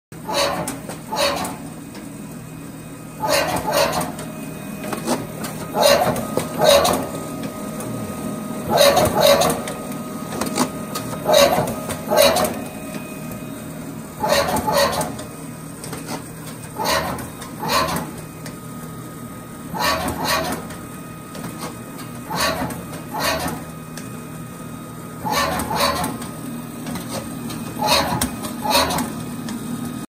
Print Plus Web - Zurko Promotions
We're printing 2019 posters for the Centreville Michigan Antique & Vintage Flea Market customers and vendors at Zurko Promotions in-house printing facility using our Duplo USA DocuCutter 330 to trim into full bleed posters to hang in antique shops, antique malls, etc. They'll be available at our first market May 12 for free.